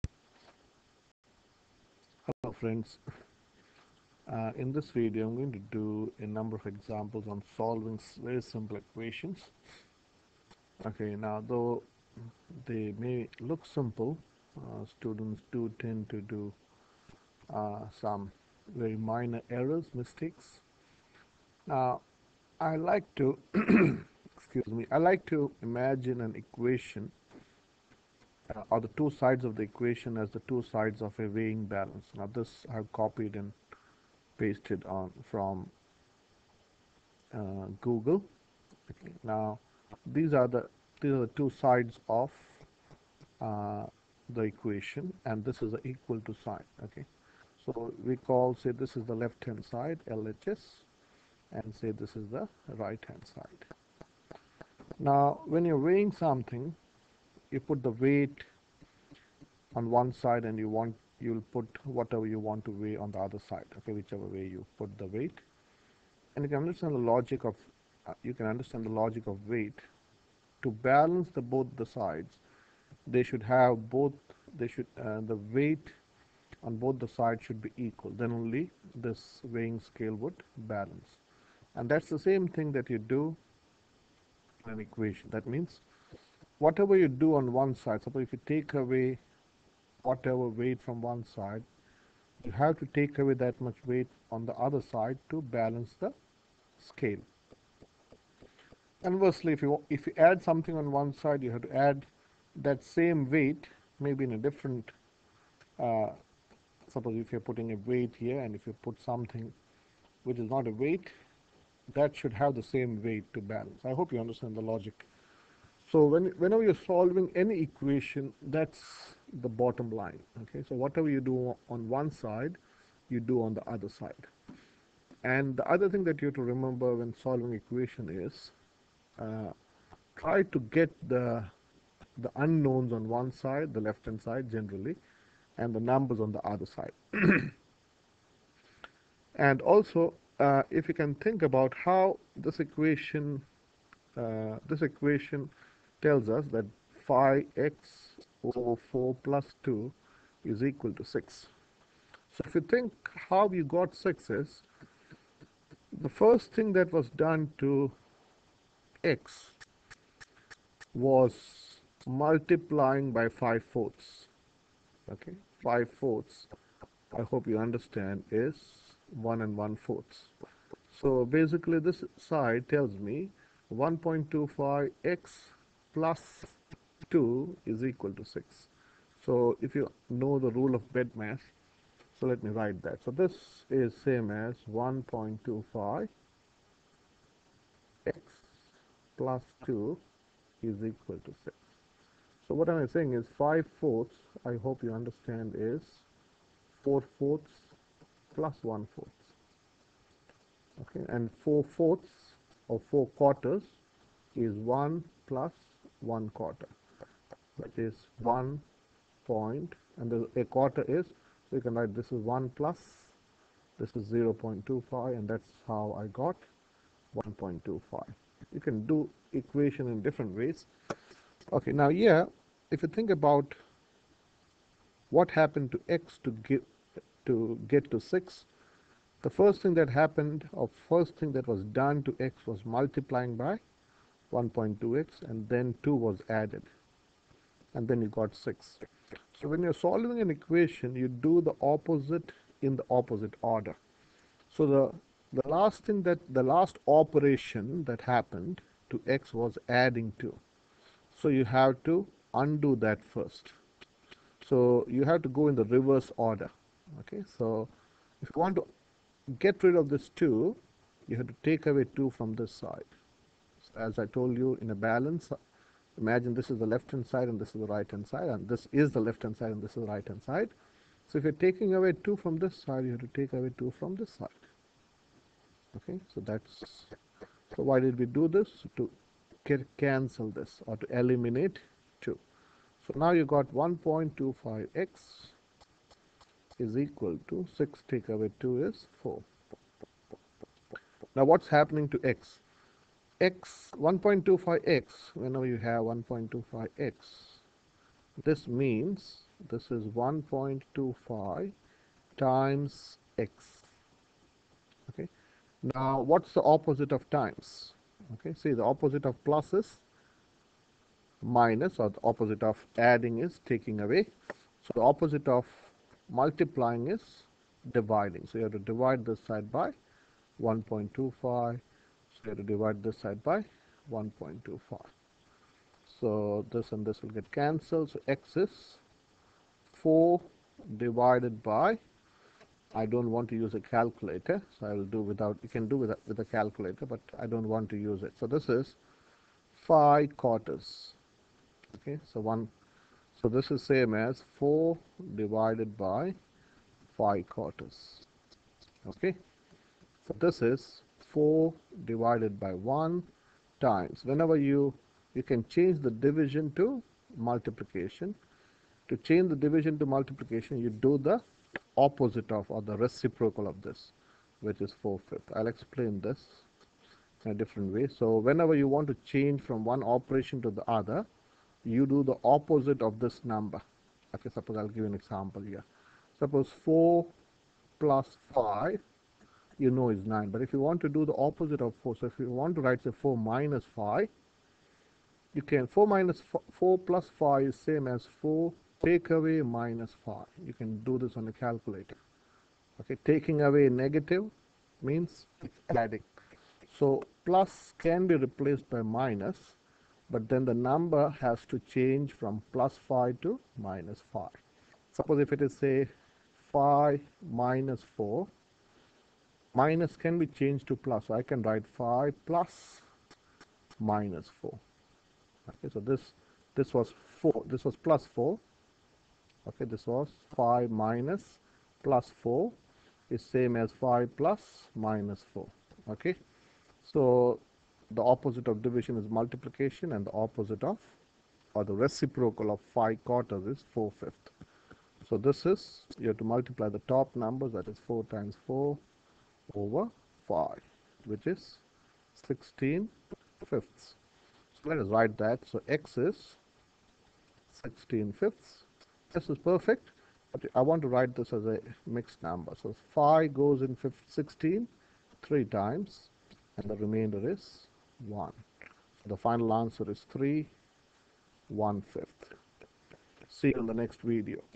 Solving equation 5x/4+2=6
In this video I have explained the logic behind solving linear equation